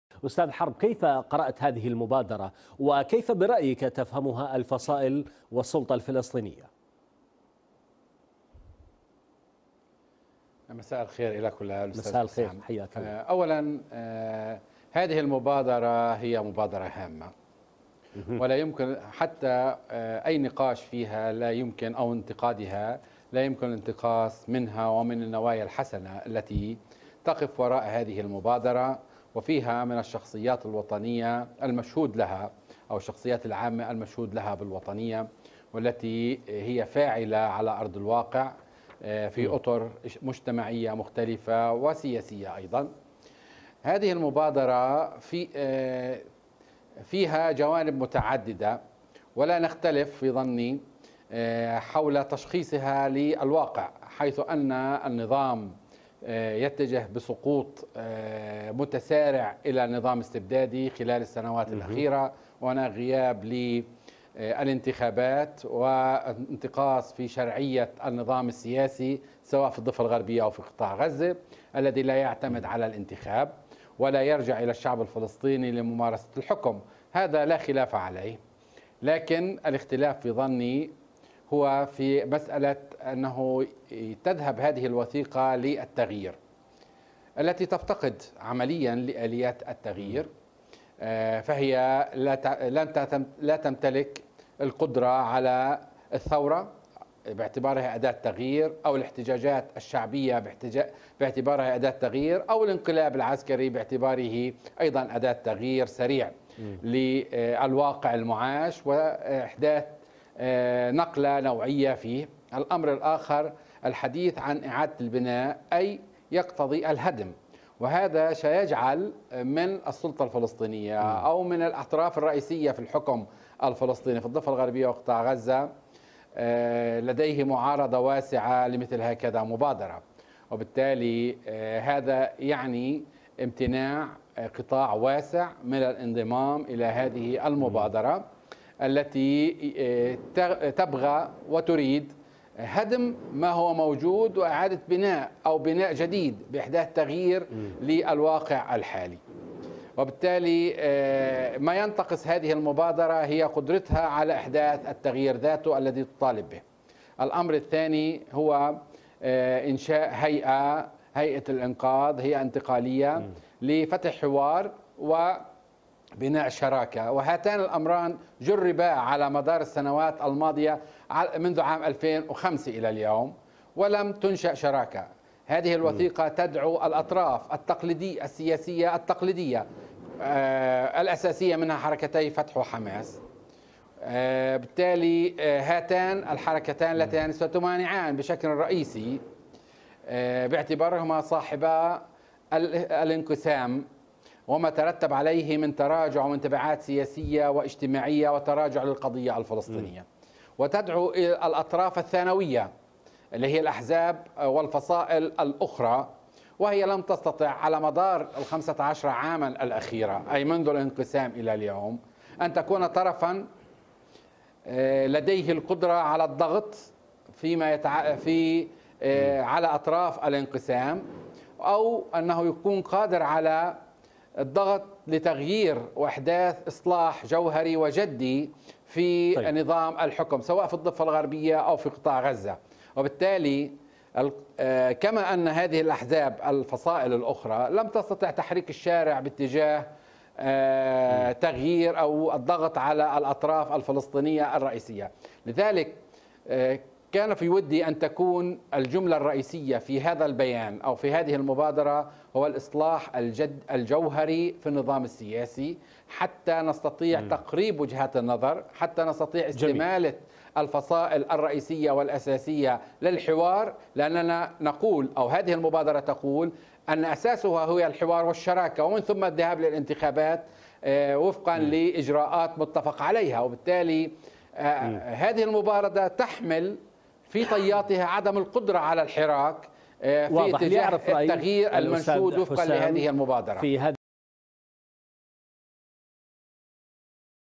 تحريك المياه الراكدة.. كيف يقيم المراقبين مبادرة الإنقاذ الوطني في فلسطين؟
أعلنت العشرات من الشخصيات الفلسطينية عن إطلاق مبادرة_للإنقاذ_الوطني_الفلسطيني وذلك عبر هيئة انتقالية للإنجاز والتغيير وإعادة البناء. وتنص الوثيقة على أن المصلحة الوطنية تقتضي تغيير النظام_السياسي بشكل واسع وعميق، وليس الدفاع عنه كما جرت العادة في العقود الماضية.
يناقش هذا الموضوع من رام الله جهاد حرب الكاتب والباحث والسياسي، ومن غزة حسام الدجني الكاتب والباحث السياسي، مع الإعلامي علاء عبود خلال